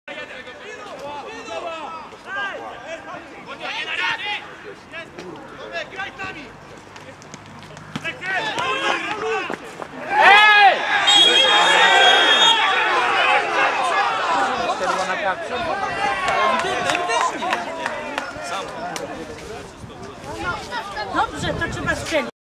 Czerwona kartka dla "Maziego" w meczu ze Spartą Jazgarzew
Czy był to faul na czerwoną kartkę? Czy zawodnik Sparty mógł wyjść na "czystą pozycję"? Czy może celowo wybrał drogę do sfaulowania go?